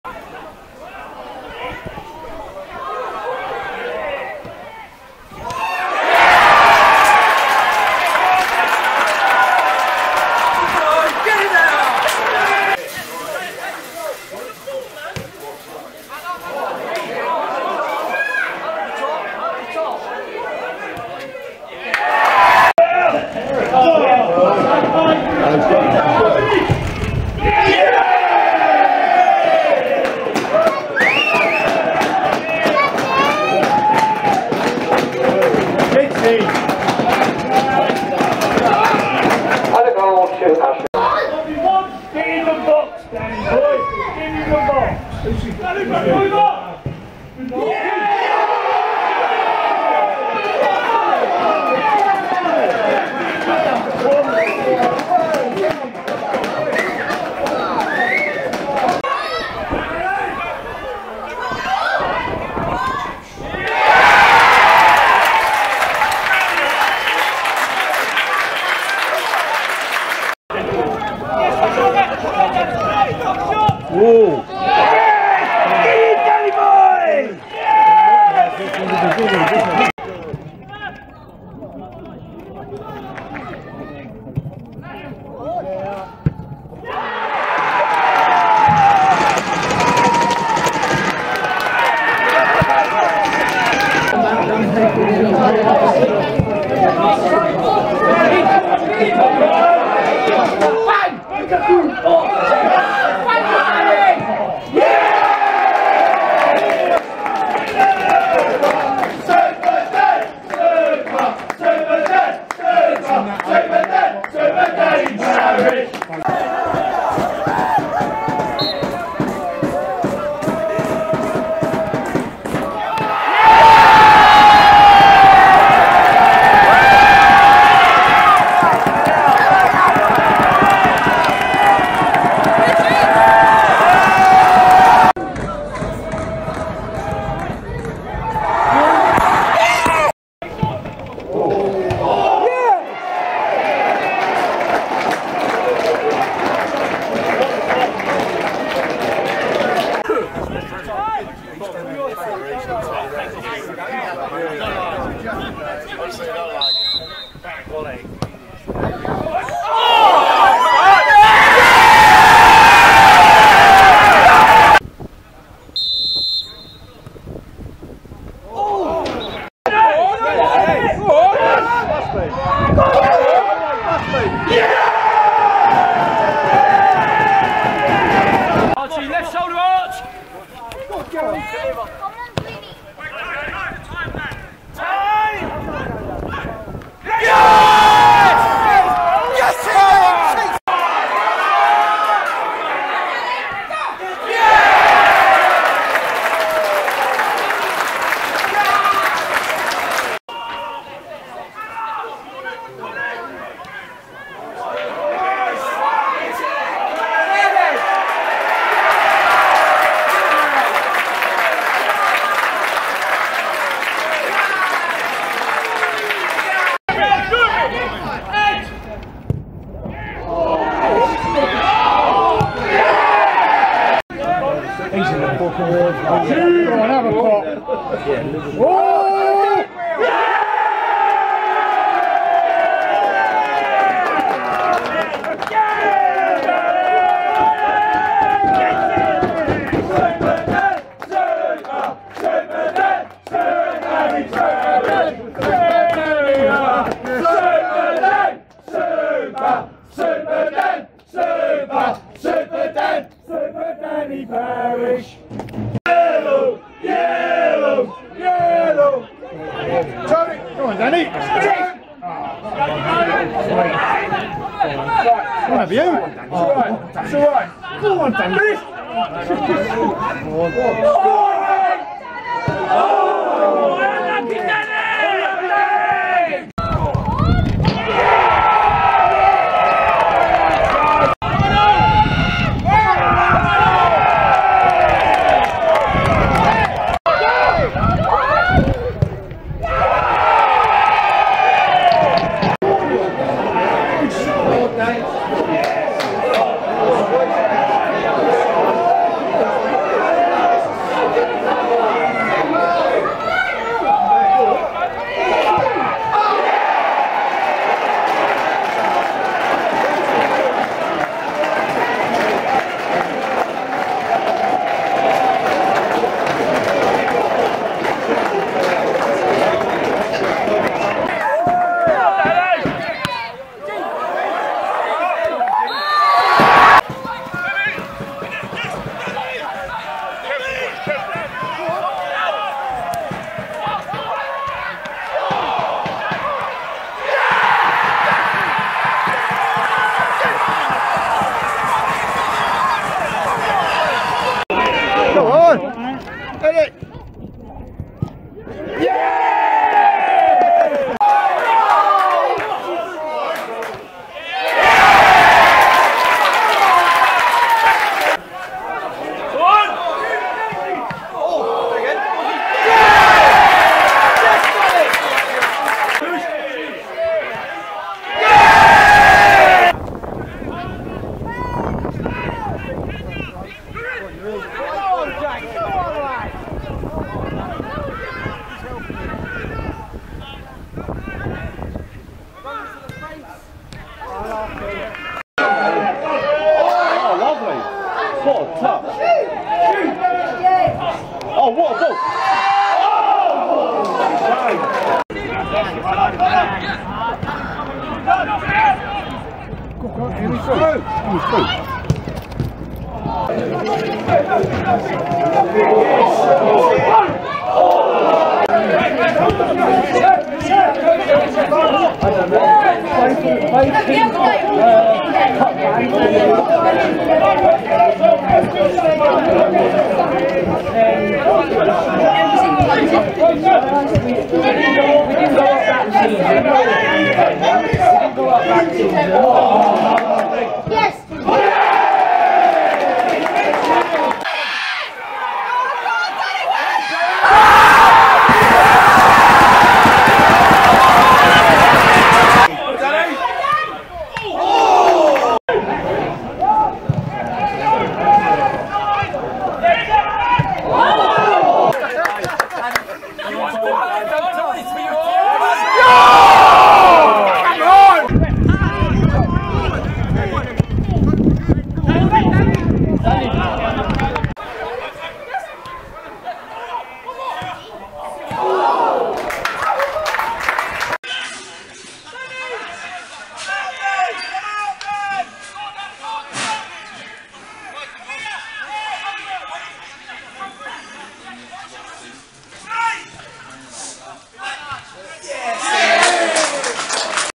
Danny Parish 2018/2019 Season
27 Goals in 27 Games (24 goals in 25 League Games) for Ashford United in the Bostik League